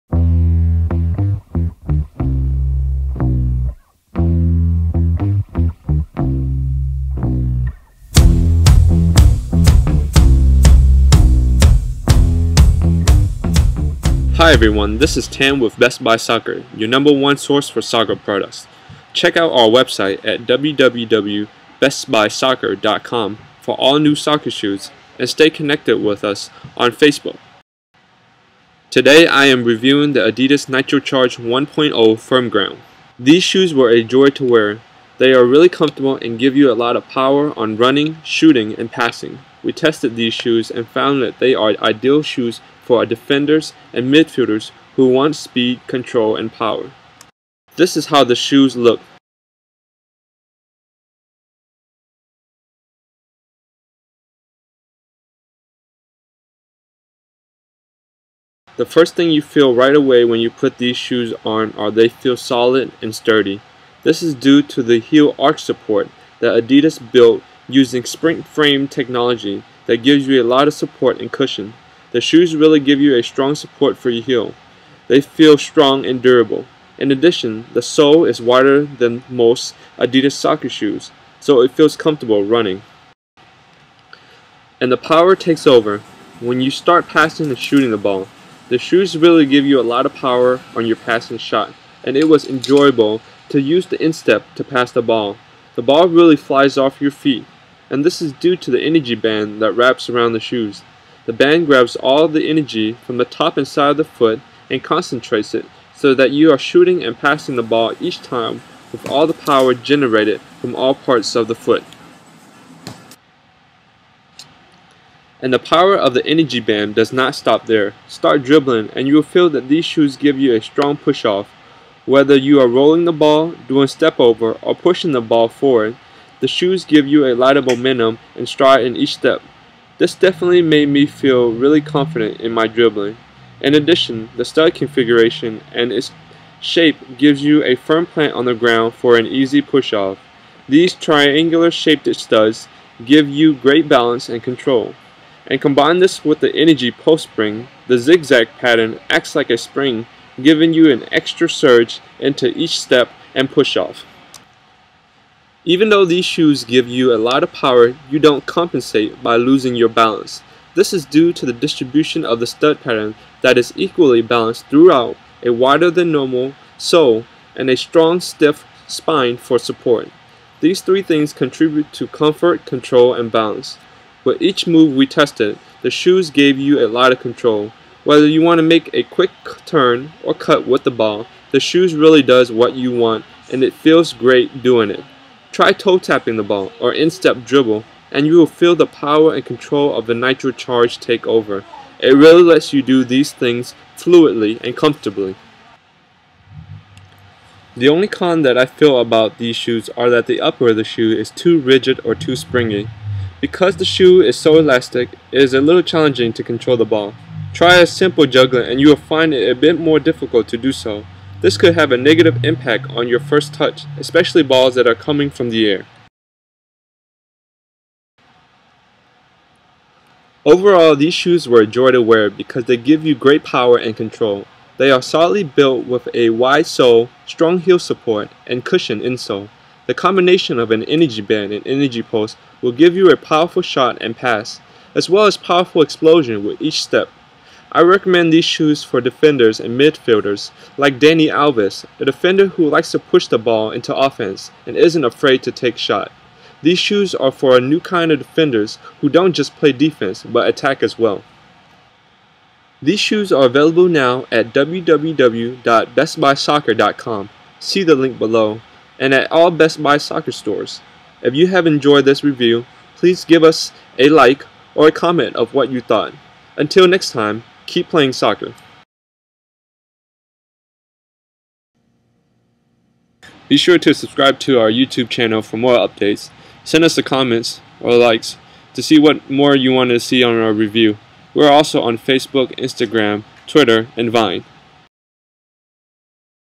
Adidas nitroCharge 1.0 FG Soccer Shoe Review
The Adidas nitroCharge 1.0 Firm Ground is a superb soccer shoe that gives a player power and control. With SprintFrame and Energy Band technology, the nitroCharge is an advance soccer shoe for the new type of soccer players.